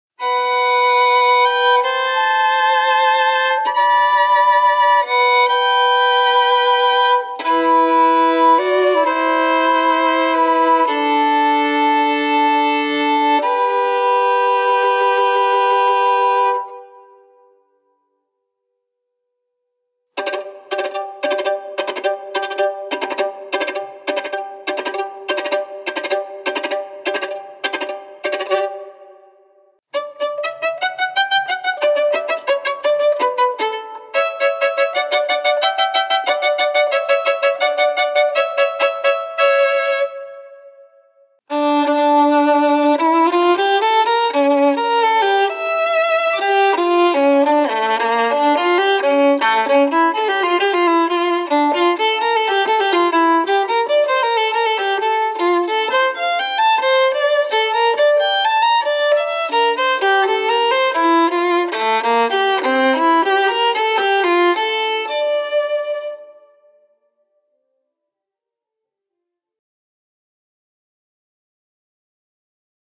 Codabow Diamond GX Alabaster Violin Bow | Gear4music demo
The Codabow Diamond GX Violin Bow with Alabaster Frog merges the traditions of bow making with the advancement of technology and modern materials. The Diamond series is regarded as one of the best Carbon Fibre bows available on the market today. The GX has been labelled as the crown jewel of Codabow, providing performance once reserved for only the best Pernambuco Bows.

This Codabow comes with a lifetime manufacturer's warranty.